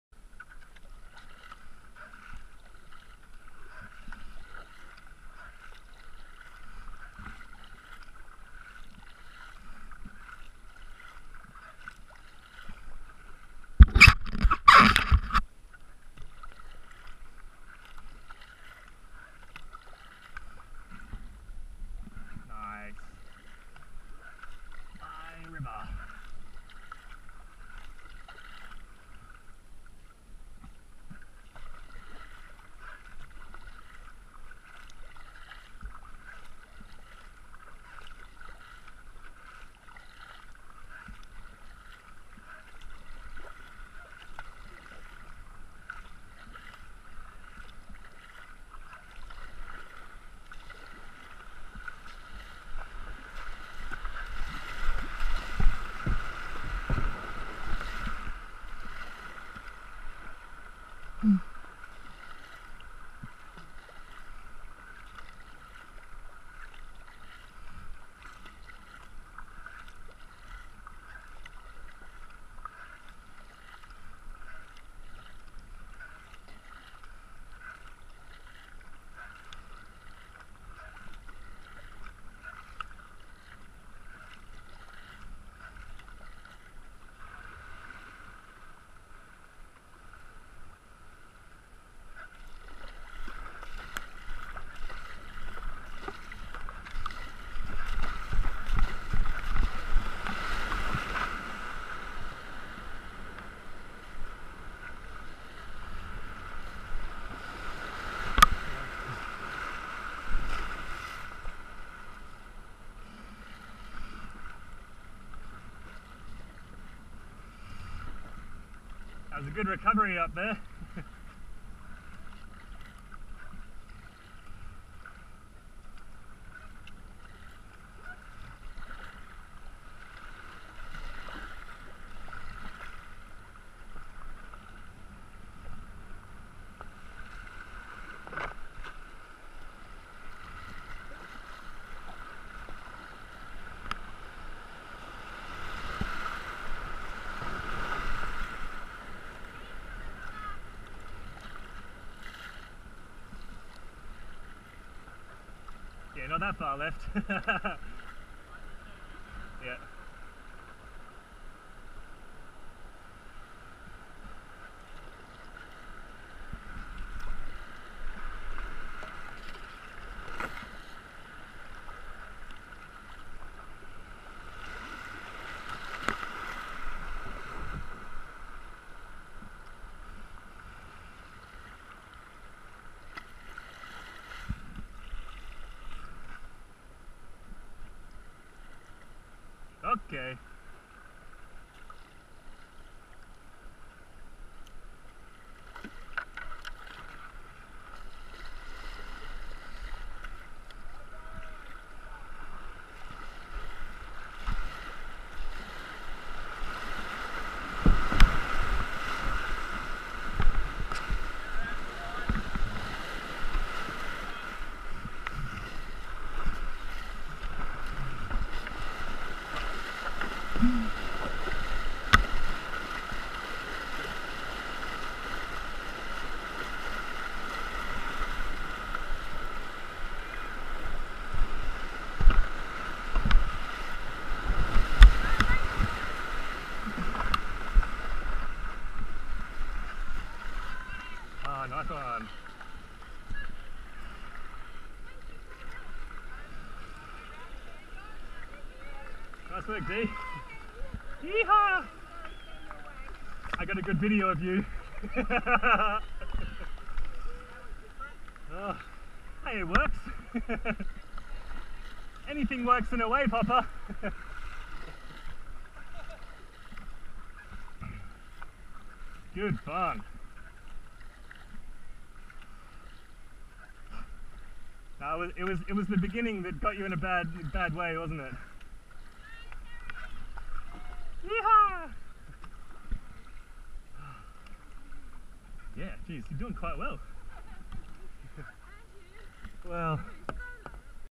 Avon Descent 2013 - 018 Day2 Syds Rapids
Helmet Cam vision from my helmet during Day 2 of the 2013 Avon Descent.
Syds Rapids is one of the big 5 rapids of the race, and is my favourite rapid after Bells.
Lots of dead trees have fallen in the river this year as there had been a bushfire in the area recently, so now there are trees to dodge while shooting this series of rapids!
Notice in the main chute at the end of the rapid, my friend in a blue boat the same as mine gets into trouble near the bottom, turning around and getting stuck on rocks; I slam on the brakes and try to reverse, then i grab a tree on the bank to try and stop - the blue boat got free just as i lost my grip on the tree, and all finished well!